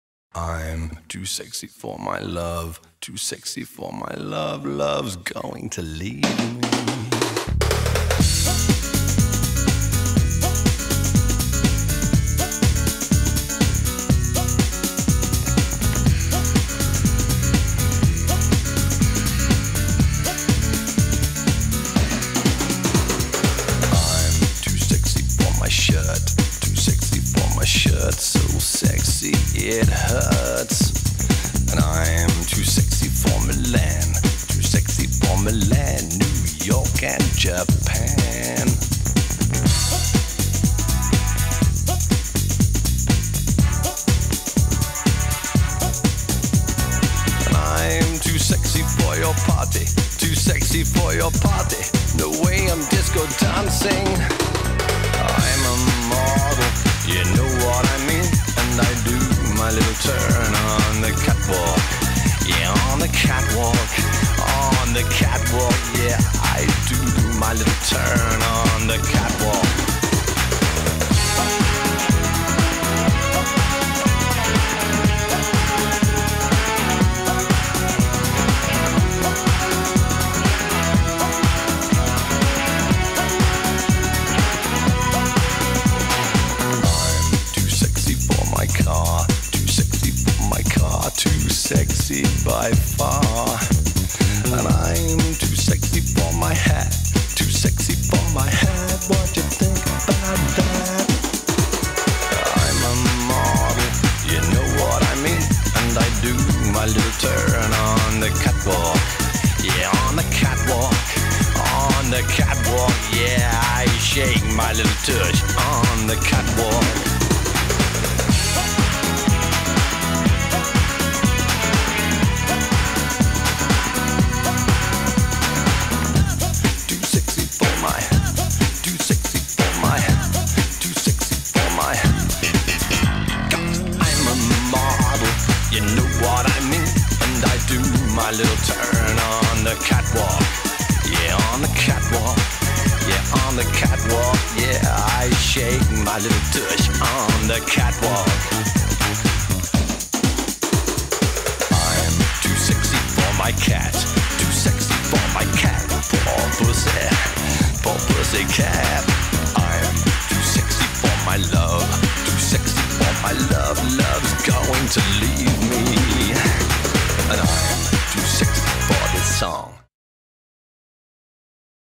Too Sexy
Sleepy time, time lapse of sketches and good music.